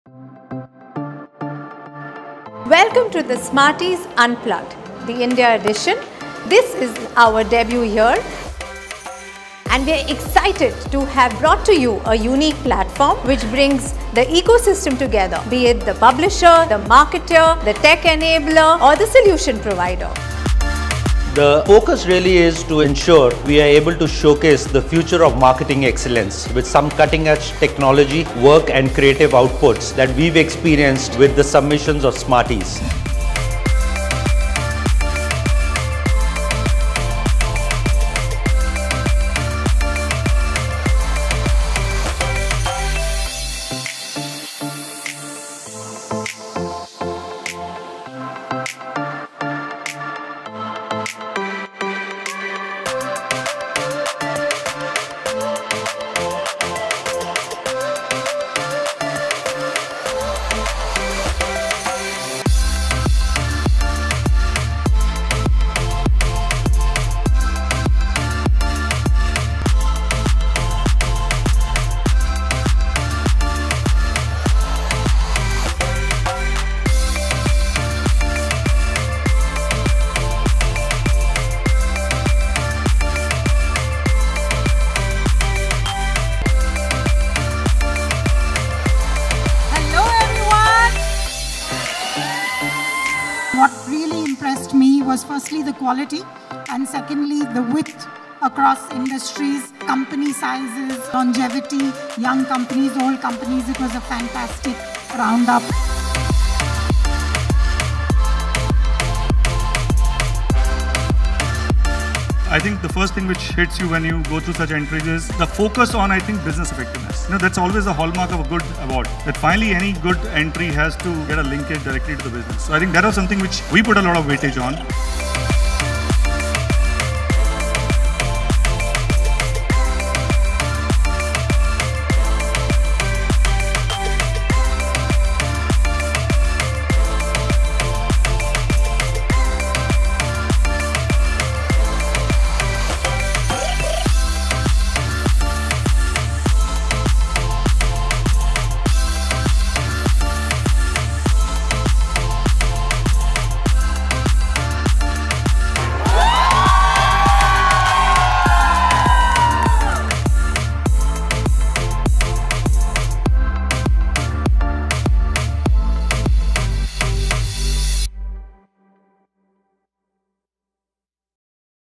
MMA Smarties India - 12th edition | Tantraa - Creating Wow Experience
🚀 Unleashing Marketing Brilliance! Join us in the latest episode of 'Unplugged,' where marketing thought leaders unite to unveil the innovative and creative campaigns shaping the future of the industry. 🌐 Dive deep into tangible case studies from SMARTIES, celebrating the secret sauce behind award-winning campaigns that not only dazzle but also deliver substantial business impact! 💡✨

And don't miss the grandeur of SMARTIES Gala Night – the pinnacle of modern marketing recognition! 🌟 Join the global phenomenon that acknowledges outstanding innovation, leading to significant business impact. 🏆✨